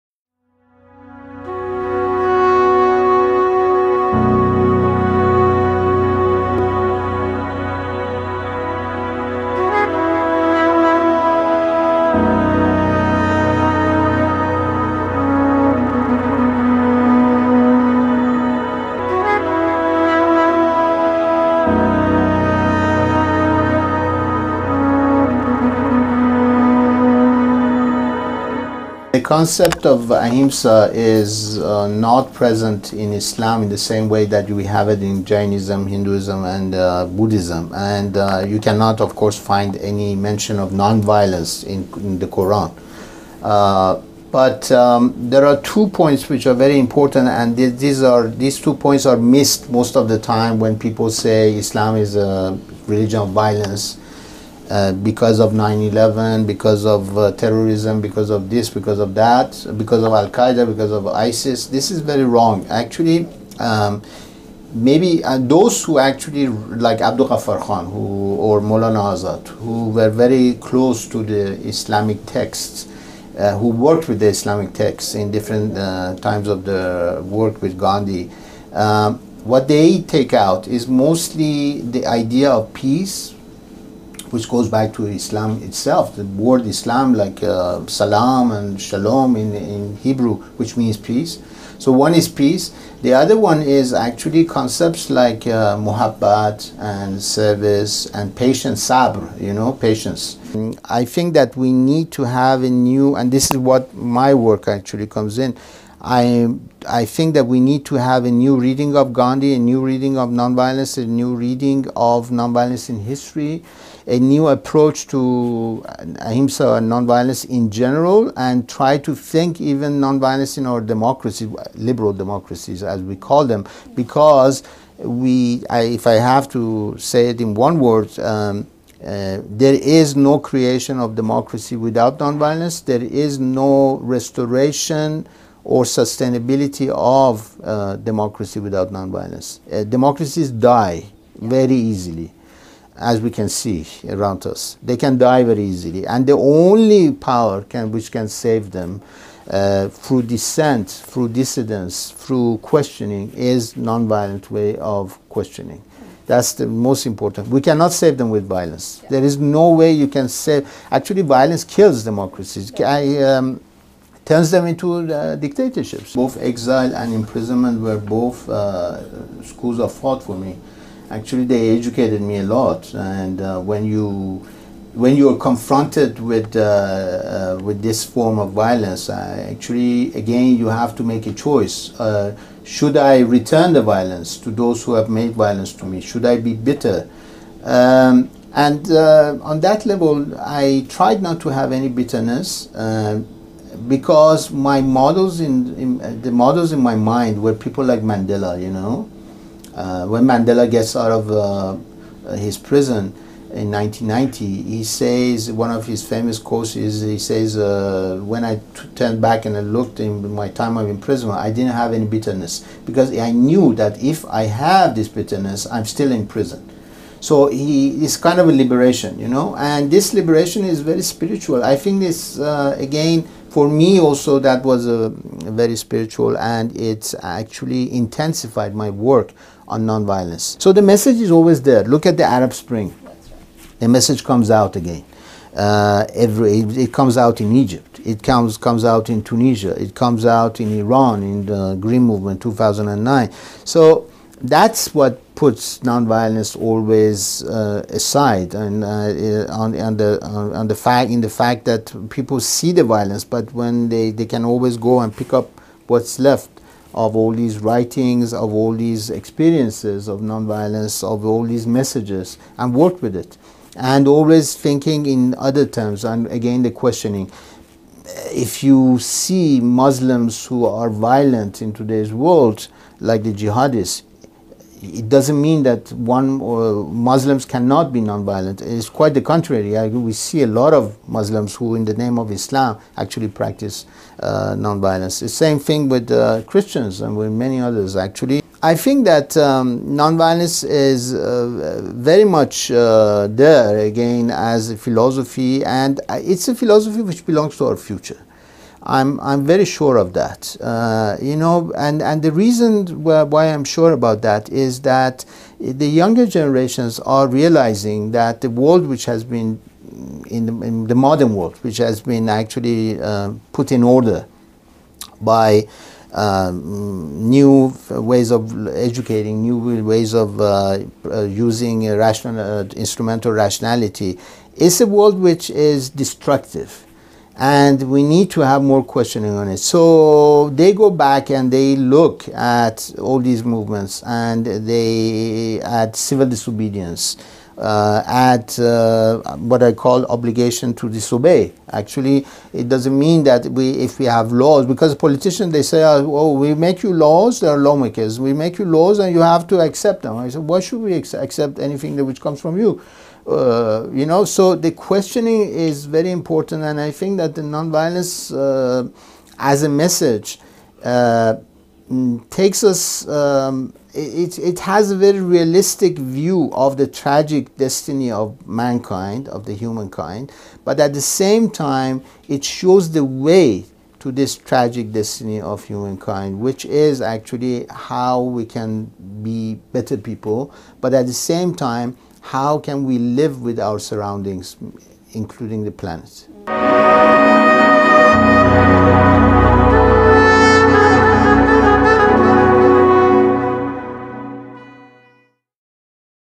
Snippets: Ahimsa Conversations # 10 Ramin Jahanbegloo
Being a political prisoner and being sent into exile strengthened Ramin Jahanbegloo’s commitment to nonviolence. An Iranian-Canadian scholar of Gandhi, Ramin describes the global presence of ahimsa as an ideal and a form of political action. He argues that it is nonviolence alone that can sustain and save democracies, and enable humans to live in balance with nature.